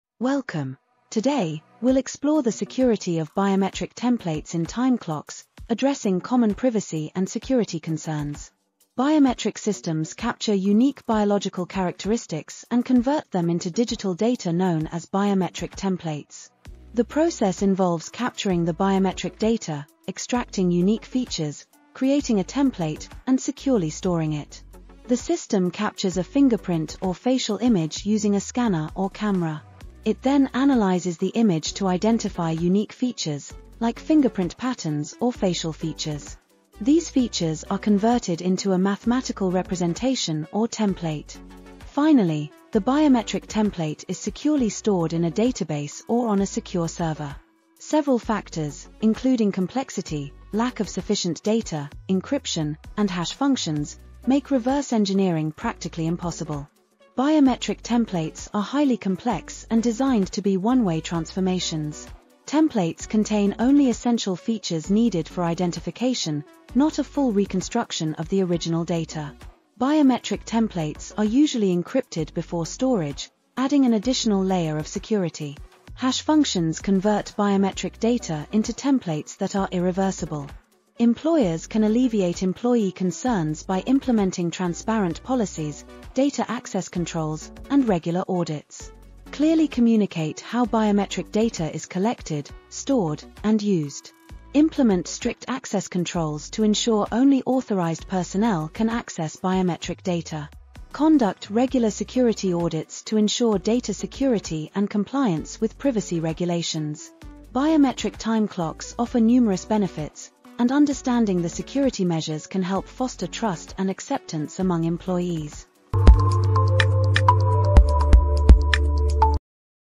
Understanding the Security of Biometric Templates in Time Clocks.
Biometric security in time and attendance systems, how they work and the security issues.